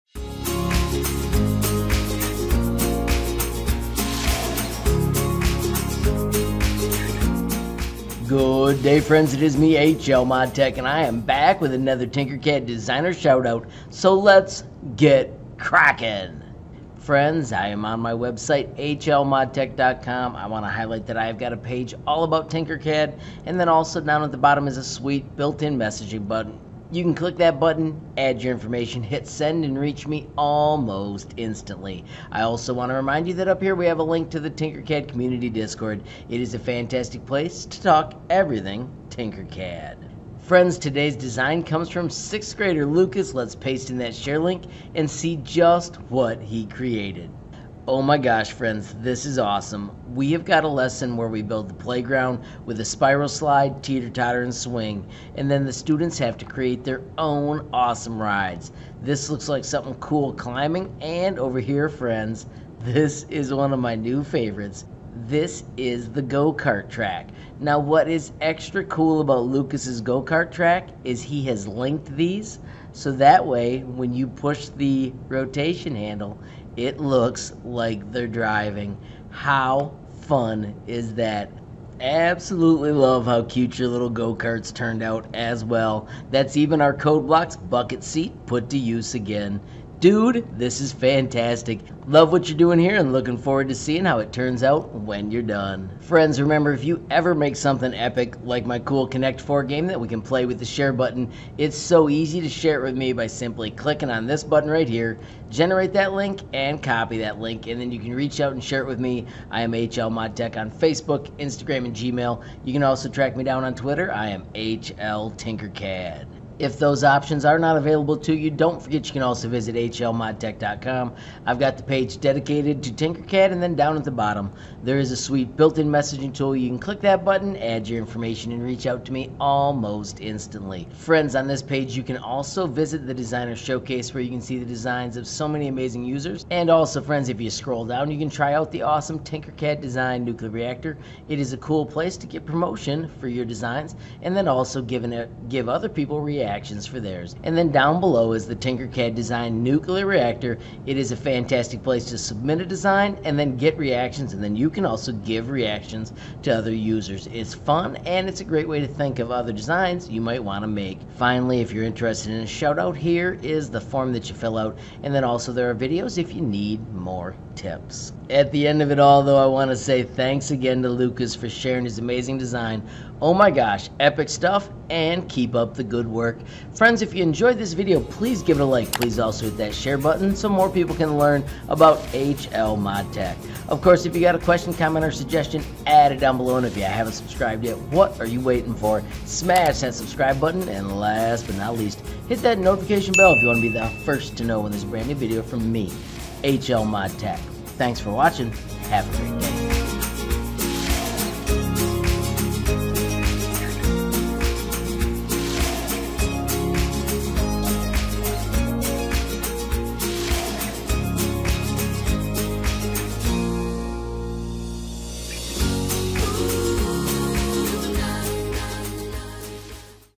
A Tinkercad Playground with Go Karts by Lucas | Designer Shoutout
Check out this Tinkercad Playground with Go Karts by Lucas. A crazy fun addition to any park you visit.